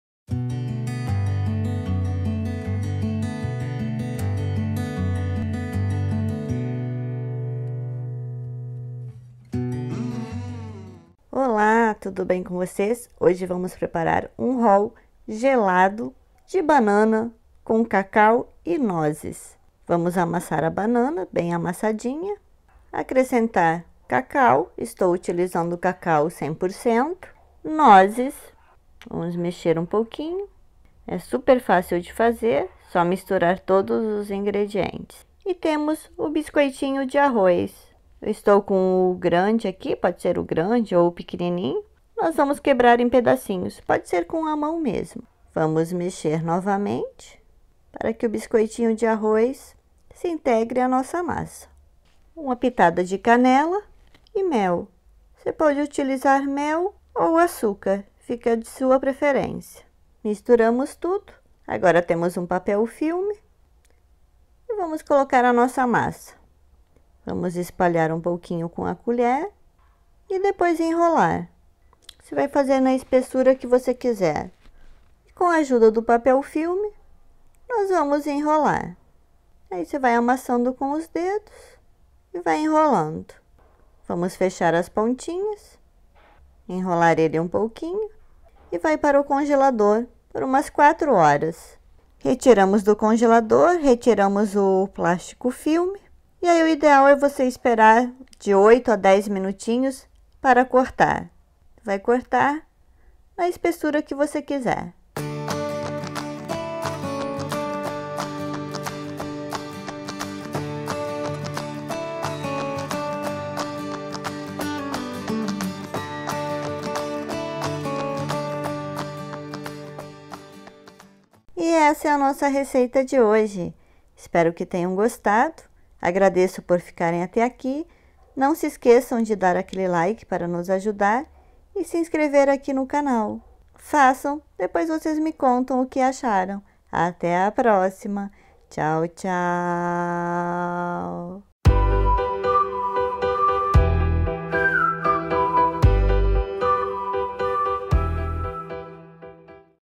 Roll Gelado de Banana, Cacau e Nozes
Roll gelado de banana, cacau e nozes com biscoito de arroz, muito fácil de fazer, só misturar tudo e levar para o congelador.
Fica uma delícia !

Ingredientes:
3 bananas grandes
1/2 xícara de cacau em pó 100%
1/3 de xícara de nozes
3 biscoitos grandes de arroz (ou 6 pequenos)
1 colher de chá de canela
1 colher de sopa de mel
Finalizamos com canela em pó e nozes